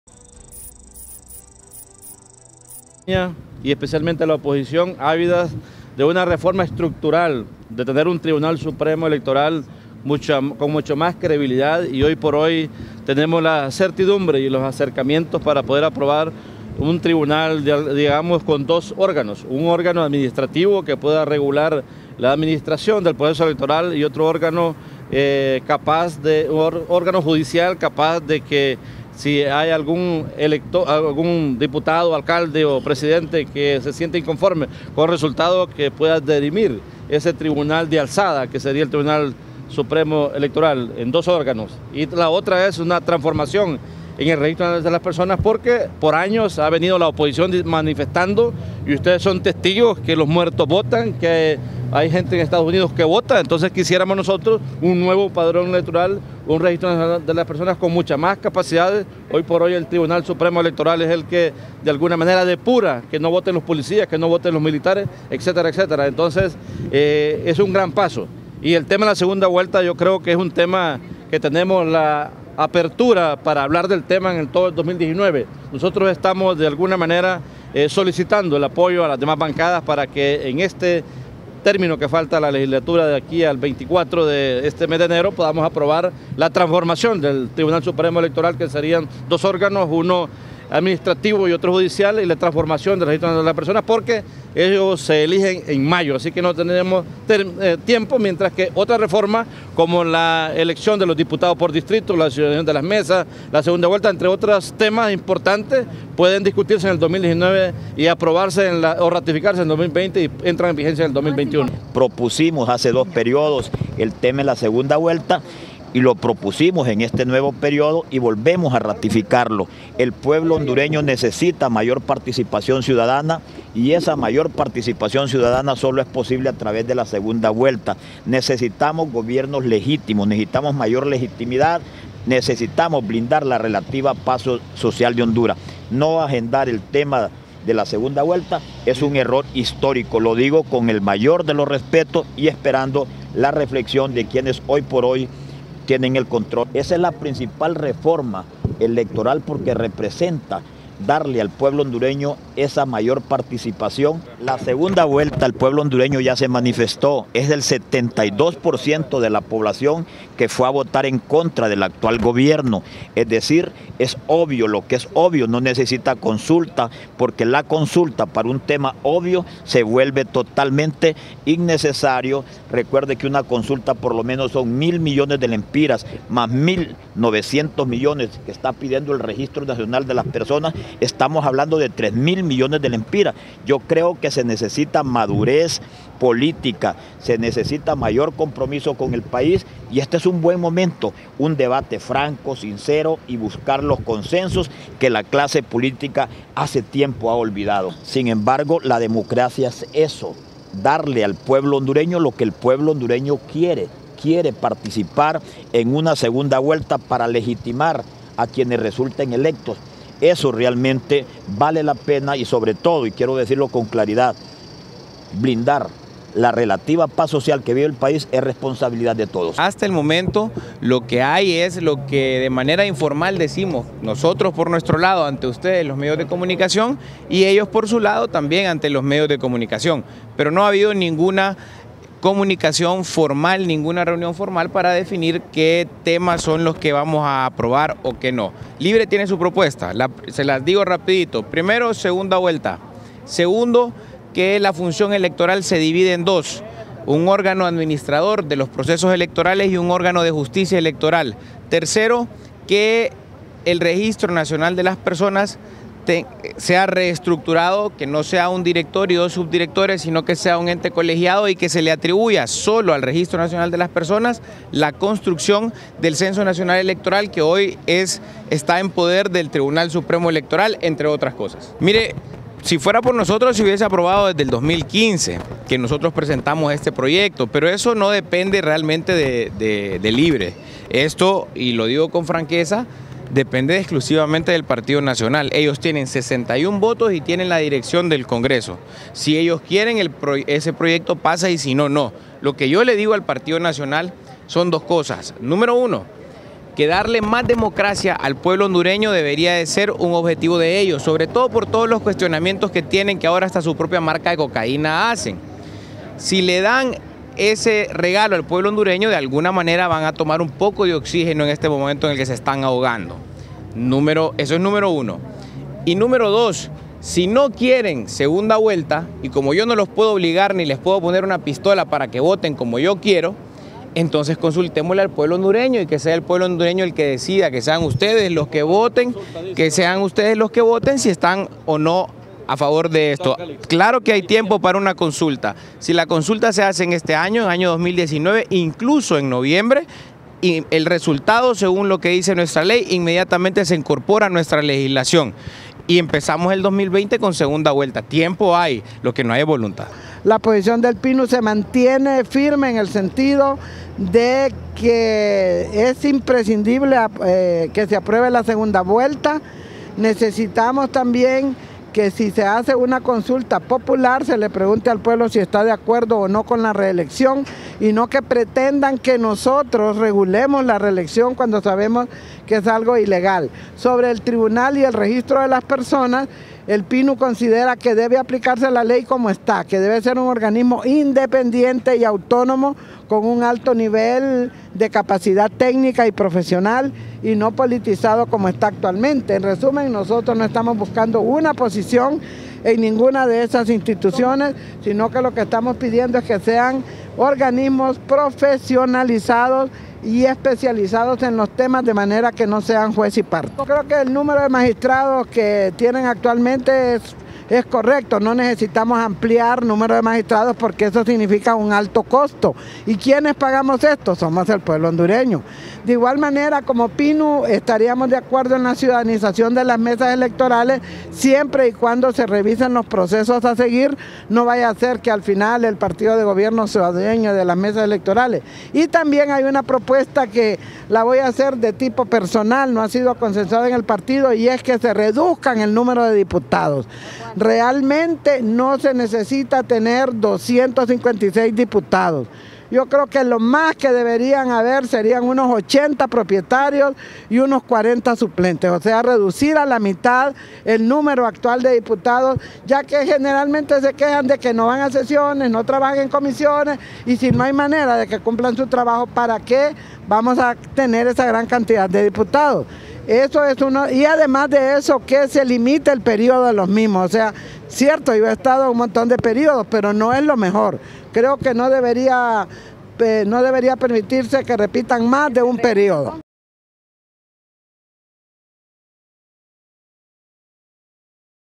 Nacionalistas insisten en que segunda vuelta electoral no es prioridad
A pocas horas de iniciarse la discusión de las reformas electorales en el Congreso Nacional, el diputado nacionalista Milton Puerto aclaró que la intención de su partido es “transformar” el Registro Nacional de las Personas (RNP) y el Tribunal Supremo Electoral (TSE)